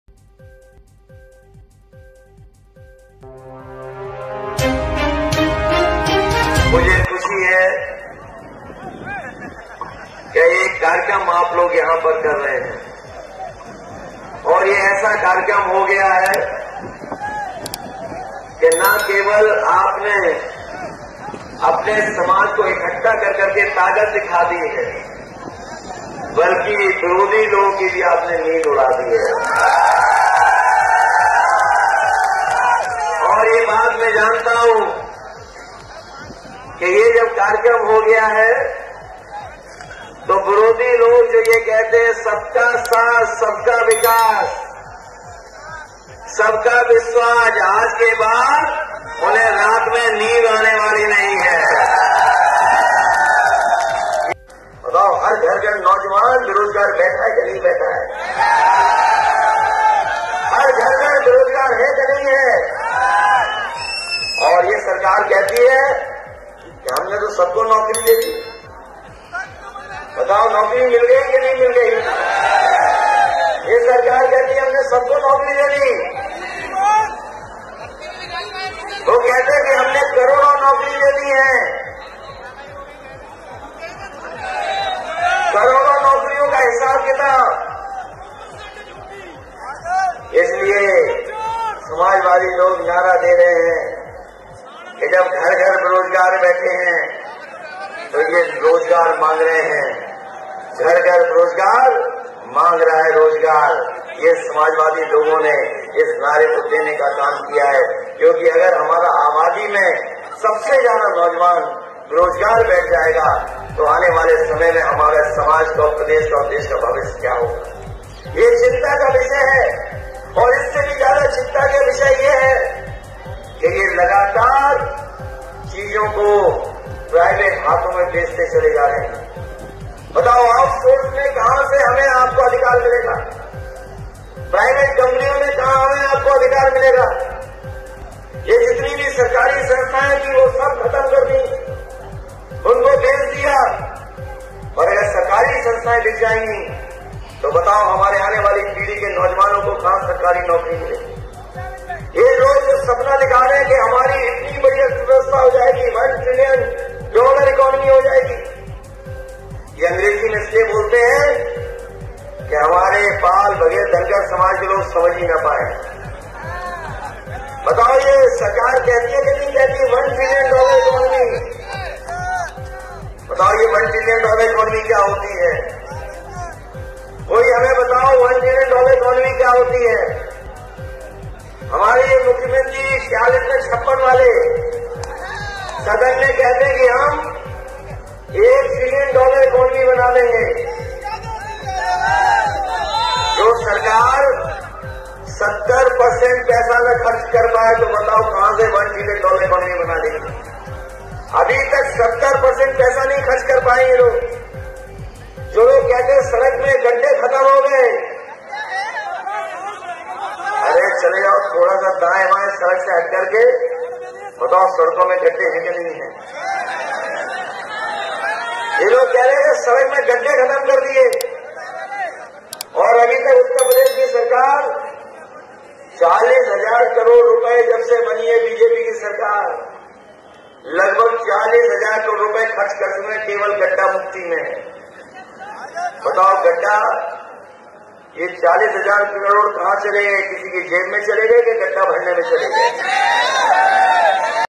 विरोधी लोगों को आज के बाद रात में नींद नहीं आने वाली।​⁠@SamajwadiPartyOfficial
यह ऐसा कार्यक्रम हो गया है कि न केवल आपने अपने समाज को इकट्ठा करके ताकत दिखा दी है बल्कि विरोधी लोगों की भी नींद उड़ा दी है। विरोधी लोग जो कहते हैं सबका साथ, सबका विकास, सबका विश्वास, आज के बाद उन्हें रात में नींद नहीं आने वाली है।"

- राष्ट्रीय अध्यक्ष अखिलेश यादव, फिरोजाबाद